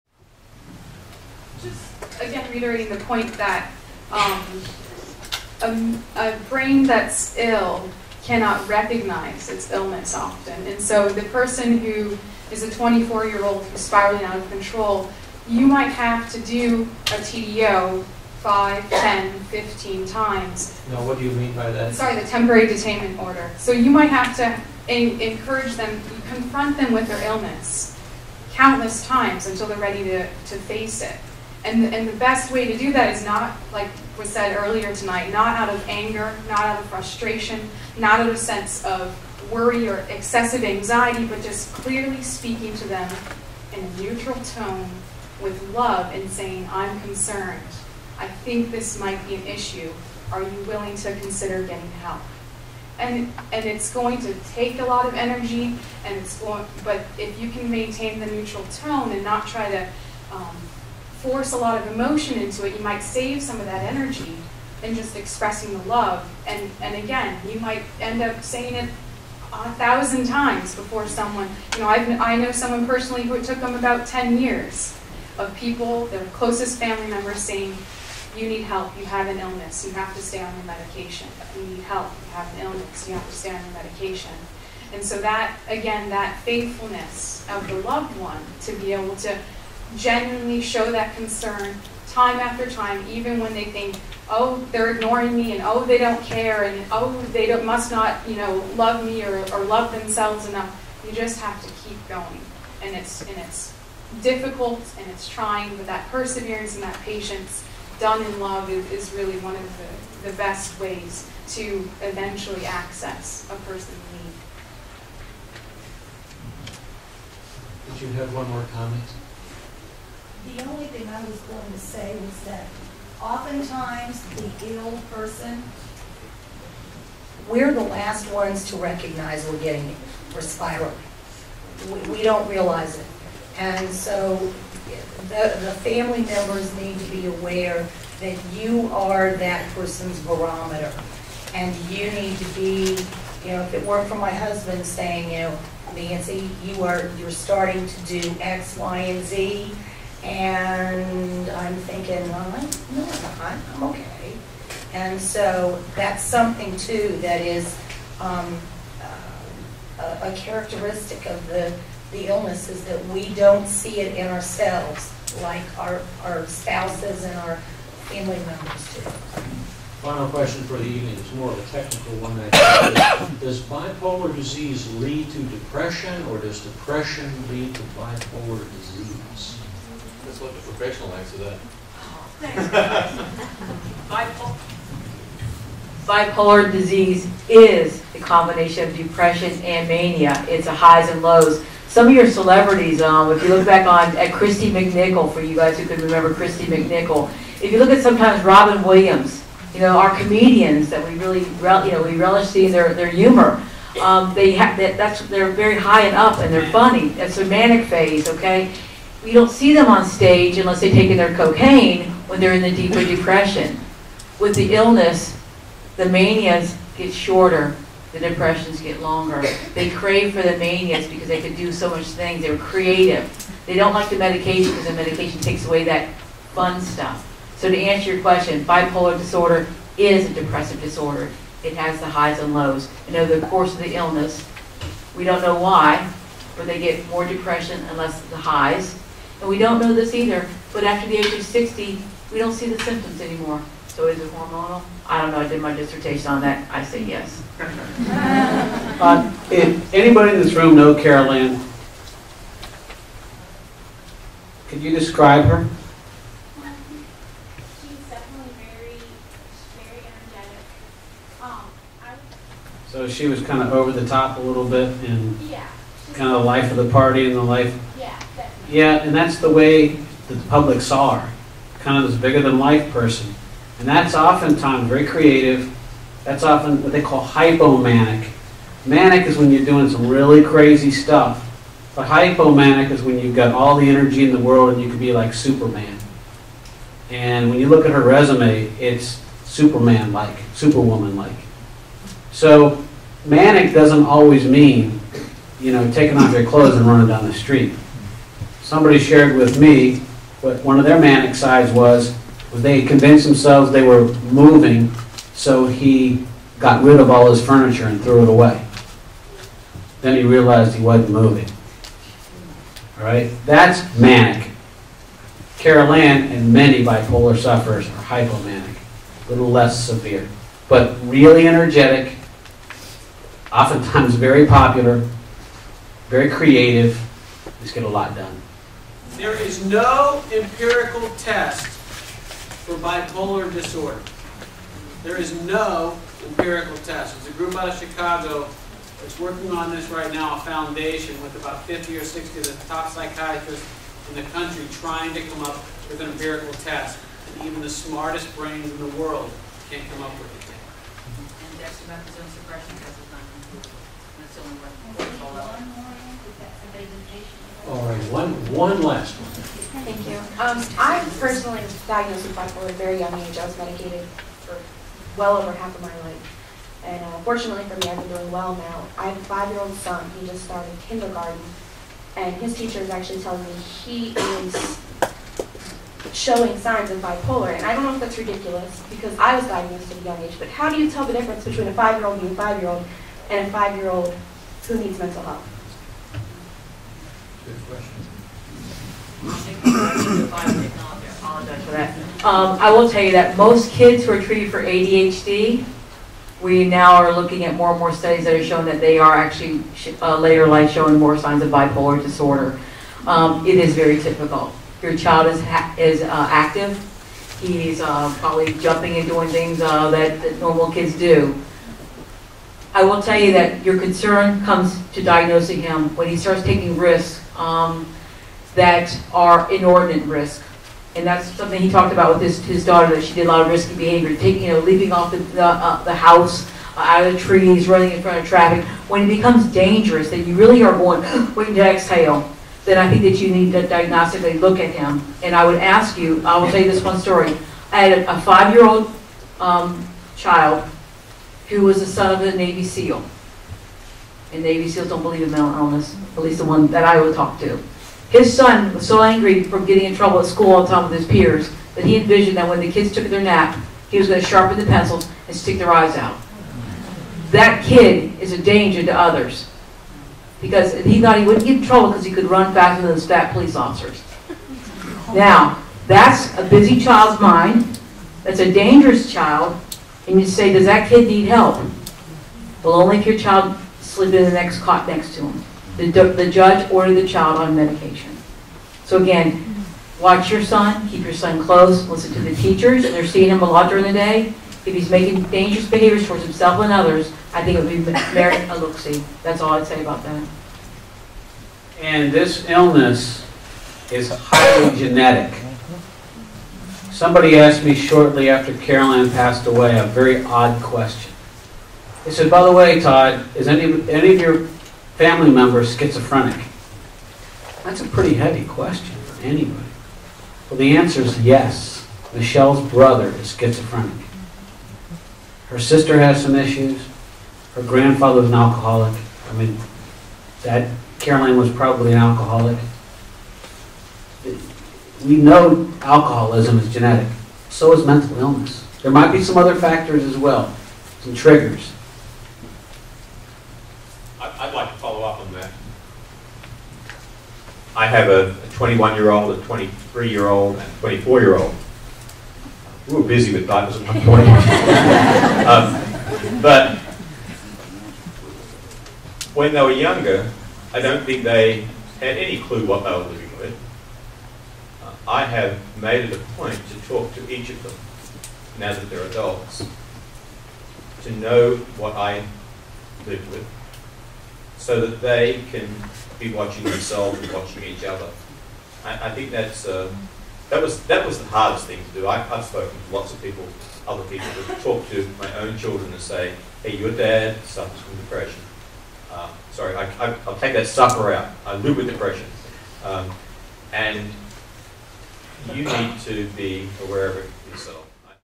GCC Panel Part 8: Q&A--families, children and depression
A Germanna Community College panel on depression and suicide takes more questions from the audience, including some dealing with impacton on families, and how young a child may be diagnosed as bipolar. October 2011 is National Depression Awareness Month. Ed Jones of The Free Lance-Star moderated the panel.